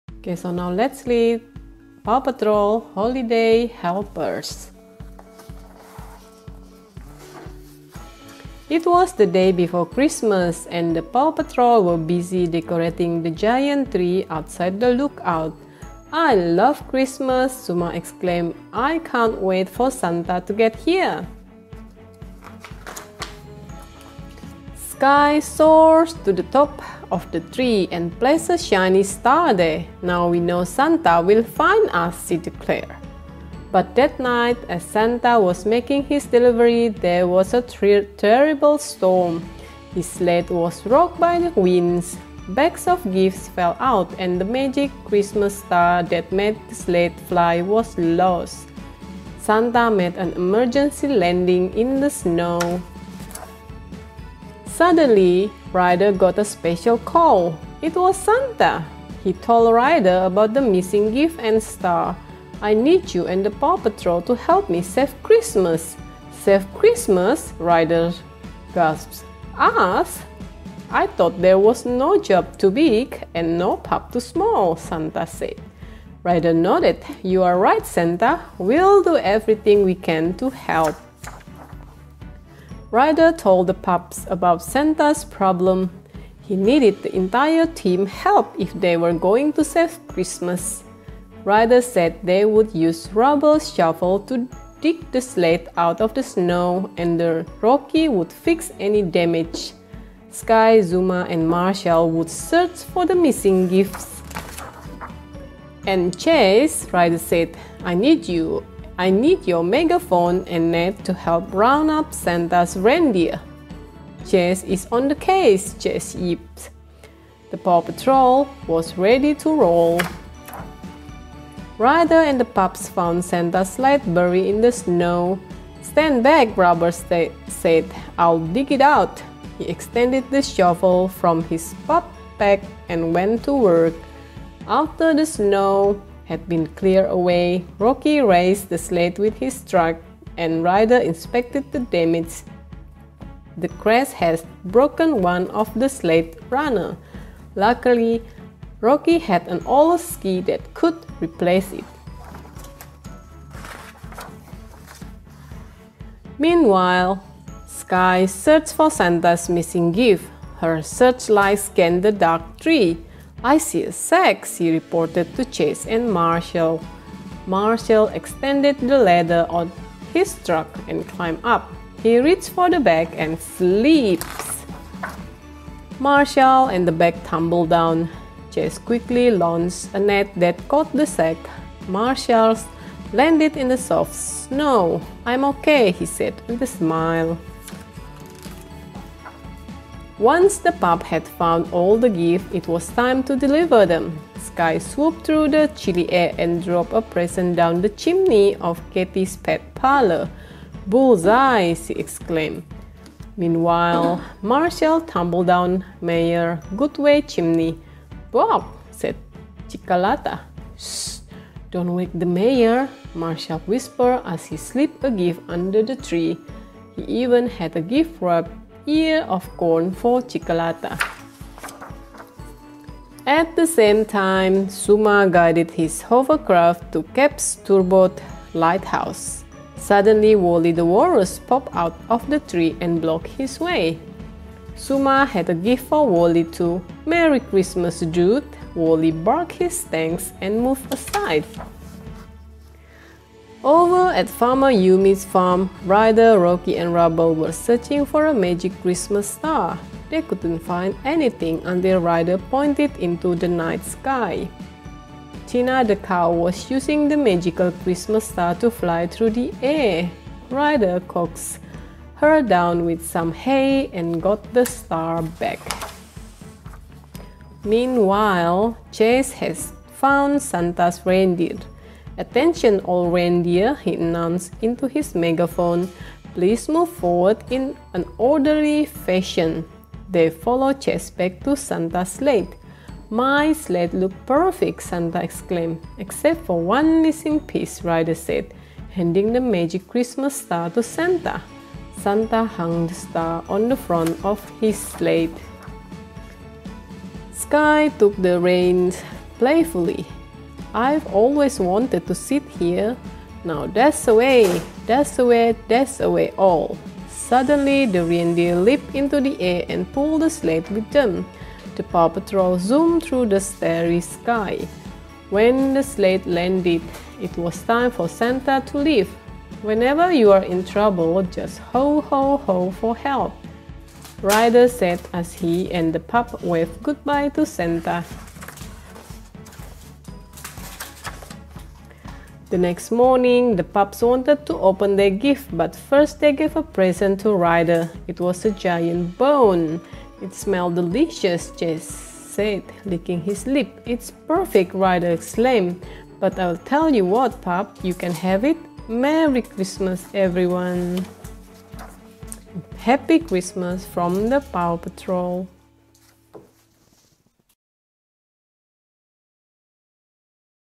Reading Nickelodeon Paw Patrol Holiday Helpers book - Christmas Children Story Time
In this video we read Nickelodeon Paw Patrol Holiday Helpers book - Christmas Children Story Time

Santa is stuck and it's up to the pups of Adventure Bay to fix his sleigh and save the day!
Can Paw Patrol perform Christmas miracle and make sure that all presents are delivered?